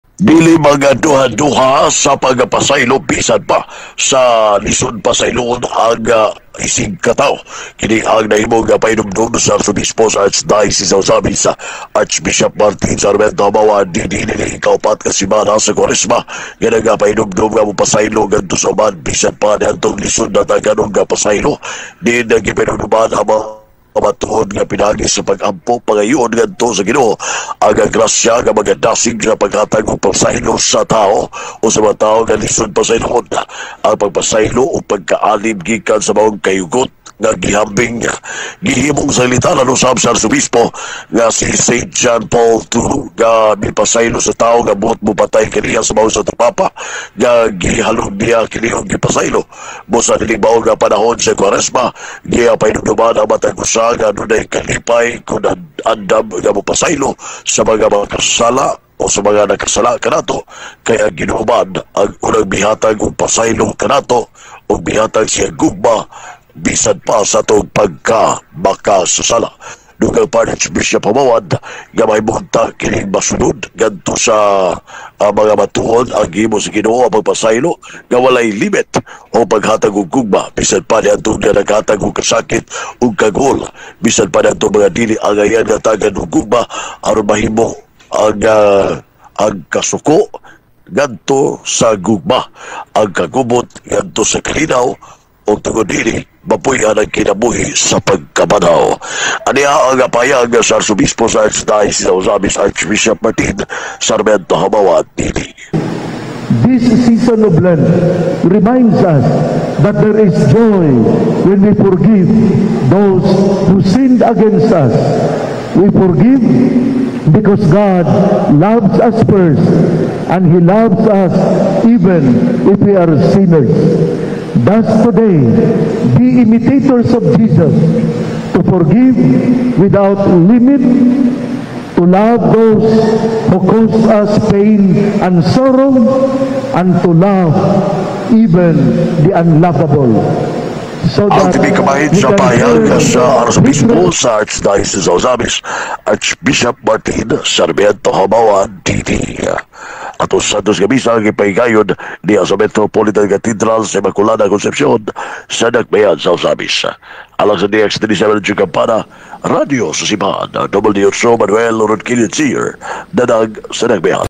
ABP JUMOAD NGADTO SA MGA MATUOHON NGA DILI MAGDUHA DUHA SA PAGHATAG UG PASAYLO SA ISIG KATAWO
ABP JUMOAD NGADTO SA MGA MATUOHON NGA DILI MAGDUHA DUHA SA PAGHATAG UG PASAYLO SA ISIG KATAWO BISAN LISUD PASAYLOON